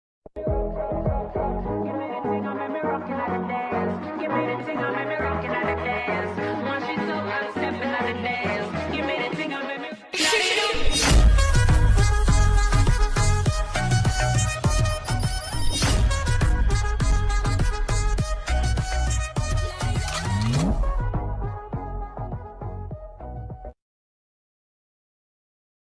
Hey Louni life!! Intro de "Notre coup de foudre" (Description)
Merci bcp pour cette intro sa me plais bcp ^^ je la poste ici à la place de l'a mettre sur ma série comme je t'avais dit car je voudrais demander un truc à mes abo du coup cette vidéo servera d'exemple

^^ voilà encore merci Louni (petit surnom hihi)